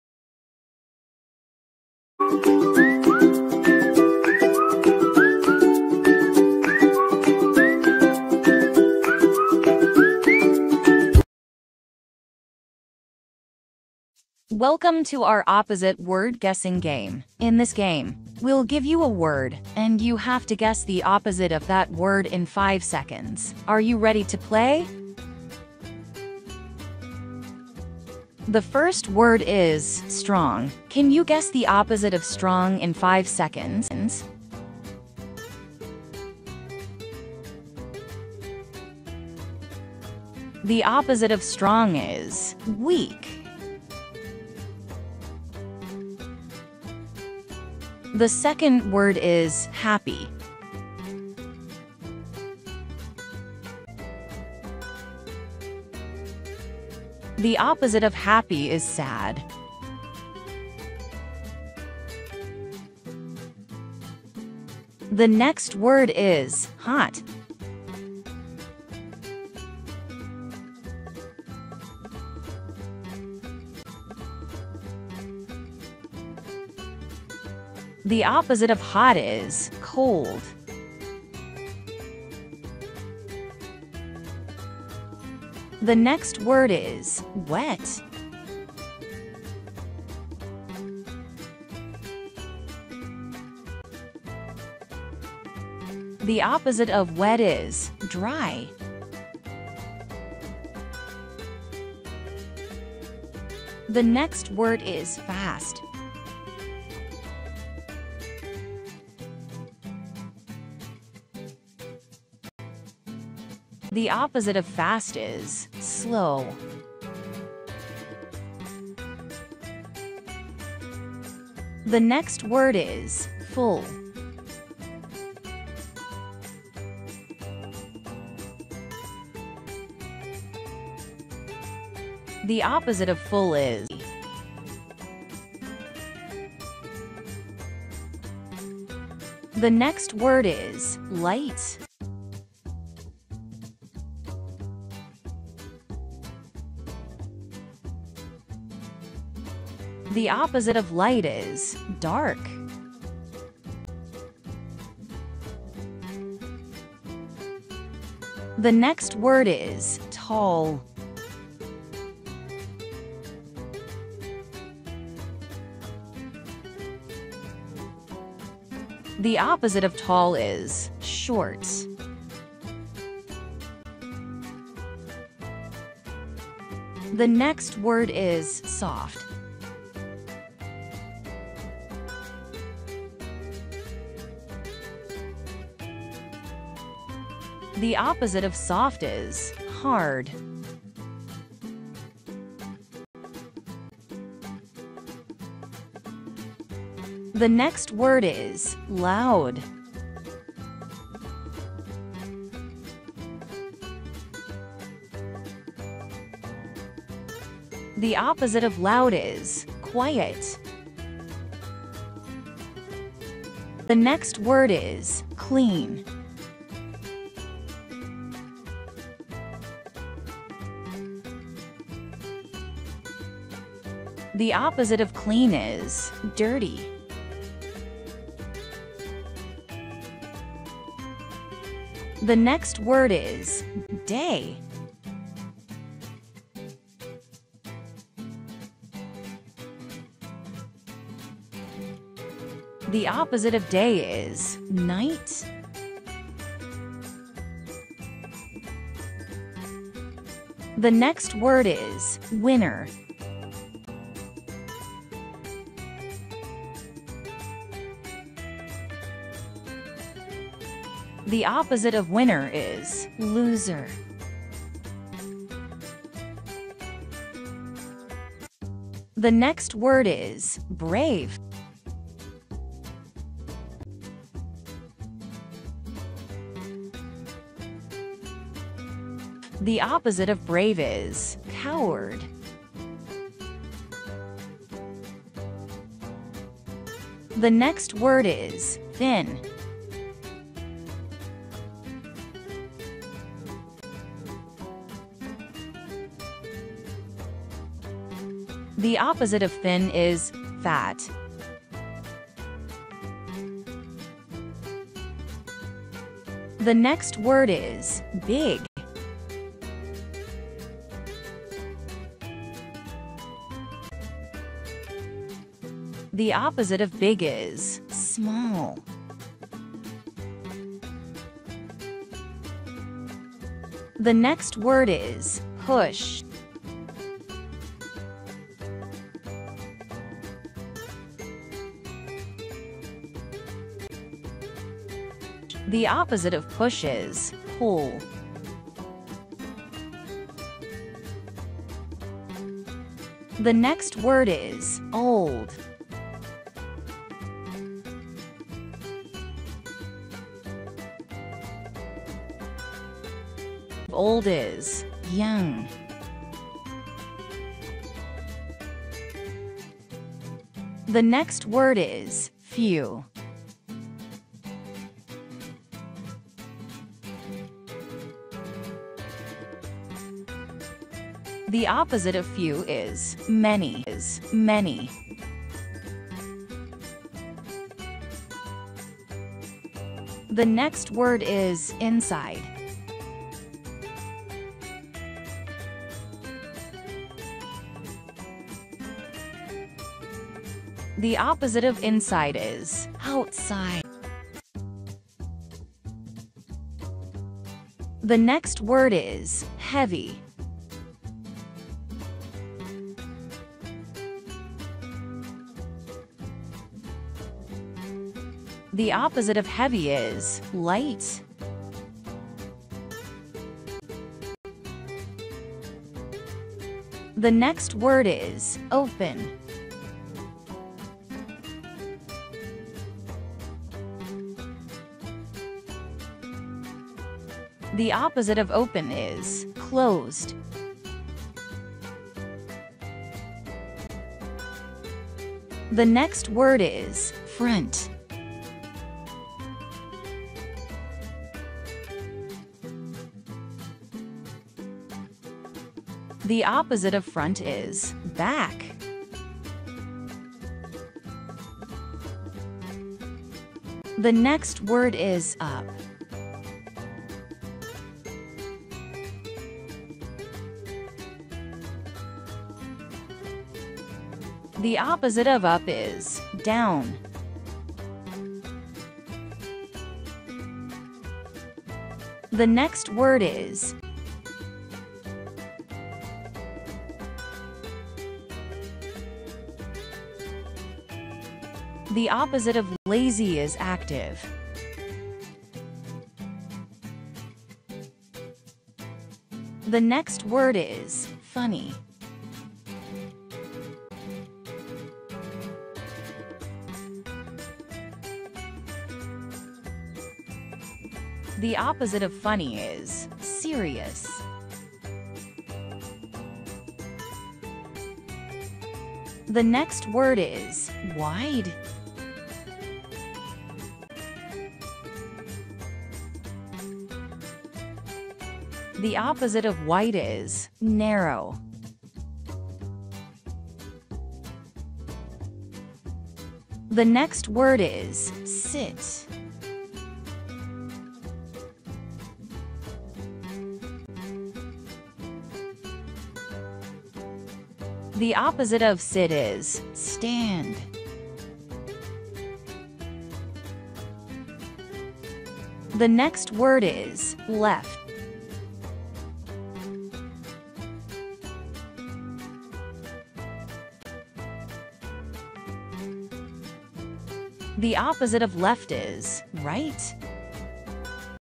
Opposite words/Opposite words in English /opposite words for kids English opposites@tinybuddies2022
Opposite words contrast each other. In other words, they are antonyms of each other. For example, black and white are complete opposites because they are the opposite ends of the spectrum for colors.
Similarly there are many example such as
Opposites for Kids
1. Good / Bad
2. Heavy / Light
3. Expensive / Cheap
4. Full / Empty
5. Wet / Dry
6. Smooth / Rough
7. Thick / Thin
8. Old / Young
9. Right / Left
10. Strong / Weak
11. Poor / Rich
12. Hot / Cold
13. Clean / Dirty
14. Quiet / Noisy
15. Right / Left